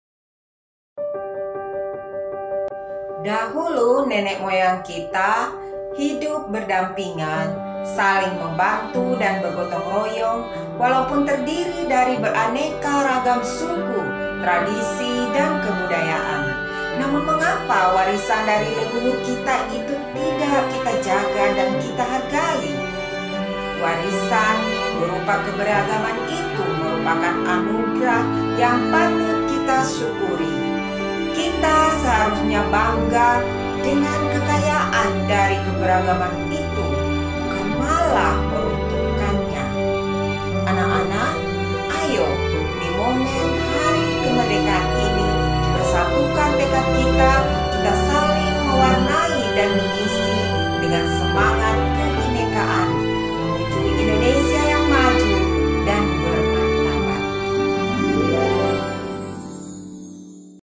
Bangga ber Bhinneka Tunggal Ika - Hasmiati H. S. Taga, S.Pd
Saya Hasmiati H. S. Taga, S.Pd Guru SMP Negeri 1 Atambua mengajak kita untuk menyatukan tekad, saling mewarnai dan mengisi dengan semangat kebhinnekaan menuju Indonesia yang maju dan bermartabat